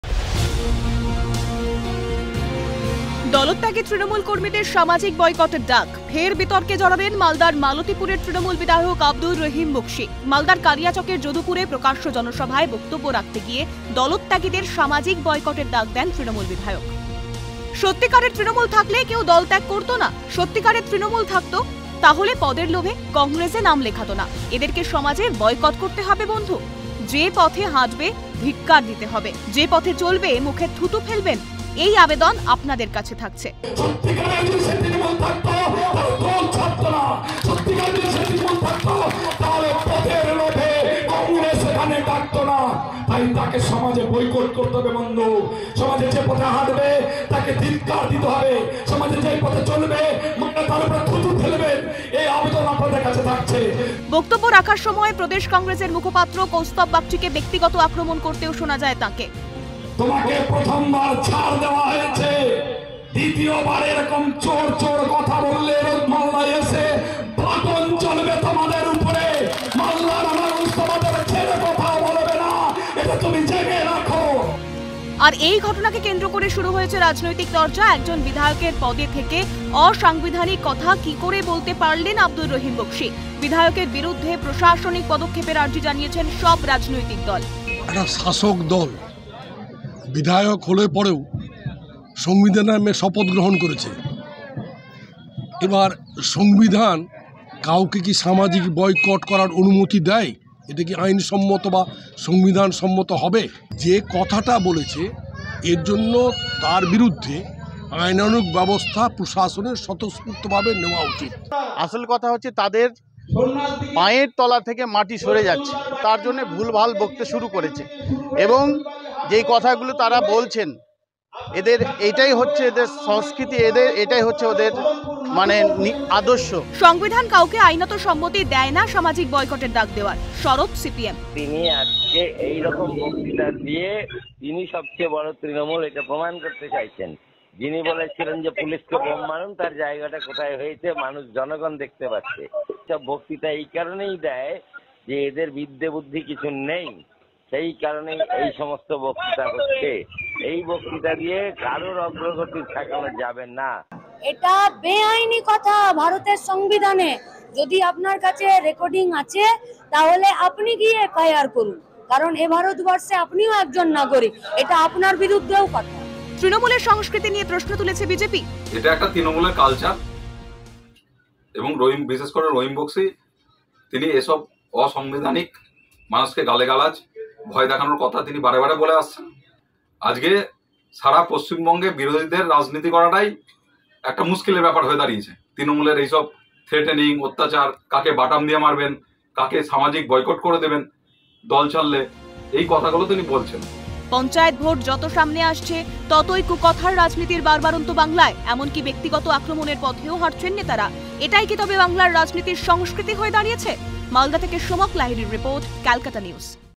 ফের কুকথার রাজনীতি | CN
ফের কুকথার রাজনীতি বাংলায়। দলত্যাগীদের বিরুদ্ধে সামাজিক বয়কটের ডাক। বিতর্কে জড়ালেন  মালদার মালতিপুরের তৃণমূল বিধায়ক আব্দুর রহিম বক্সী।